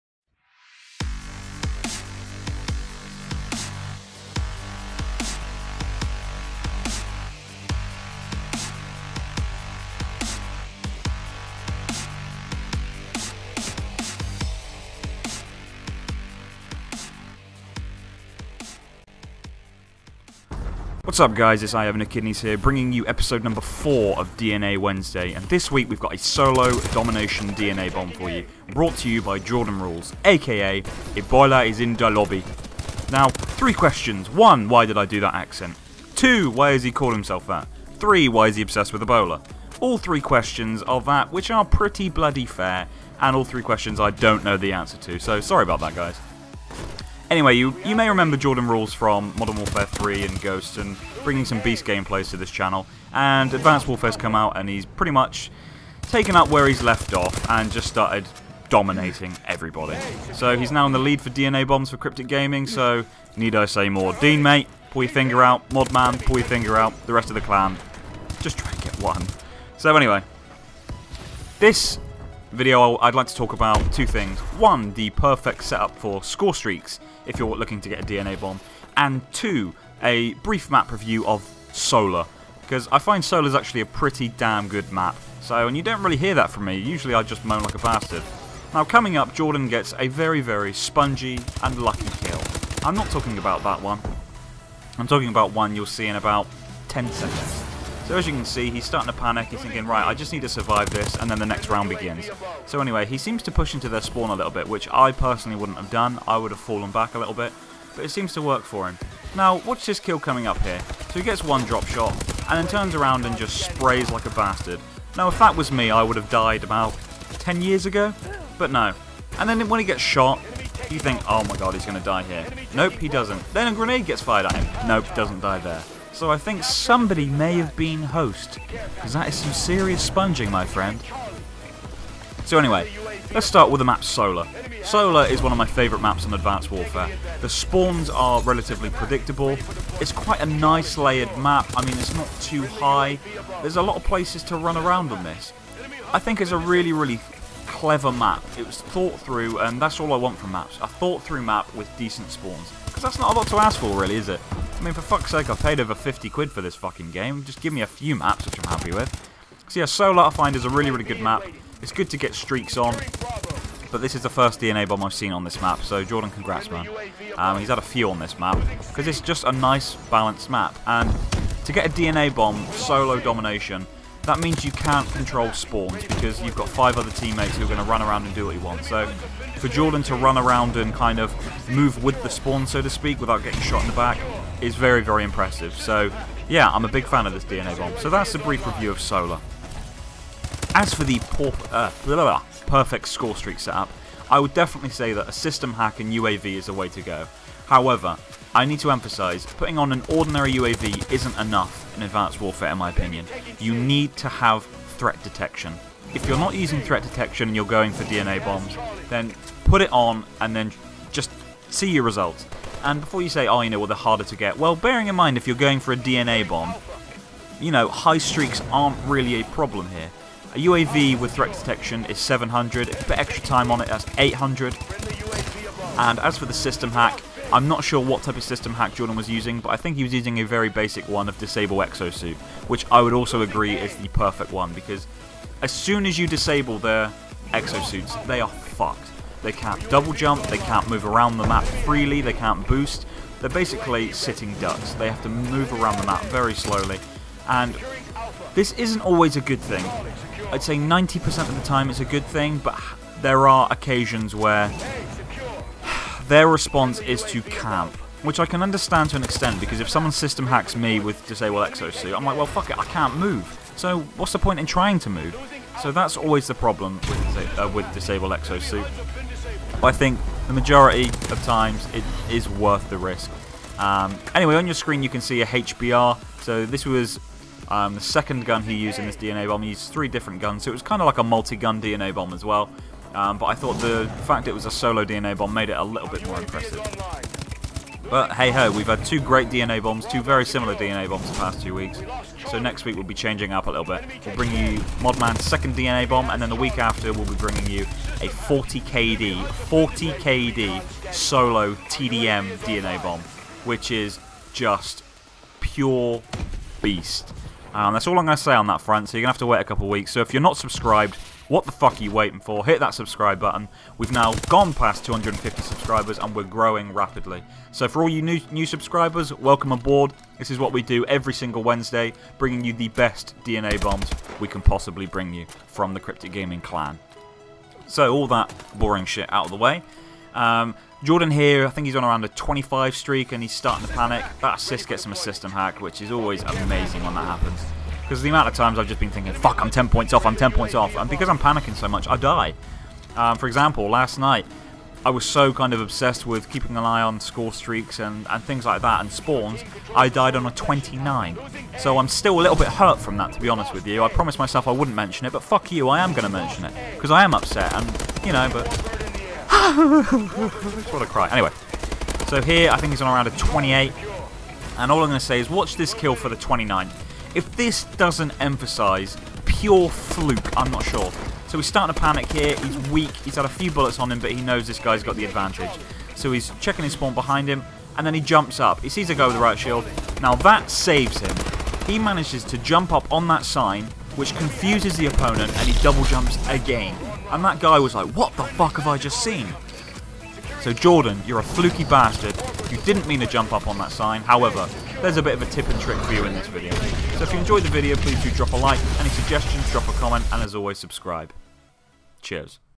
DNAWednesday: Solo Domination DNA Bomb! | Best Scorestreaks To DNA Bomb!
This is Episode #4 of CRYPTiCK GAMiNG's WEEKLY series: DNAWednesday! Want to see regular DNA Bombs?

In this episode, we have a solo, multi-gun DNA Bomb on Domination, Solar. This gameplay contains lots of sponging, a whole lot of luck and even more general quality! Watch and find out more!

We also give you the BEST SCORESTREAK SETUP for getting DNA Bombs - is this what you expected? We also give you a brief MAP REVIEW of SOLAR!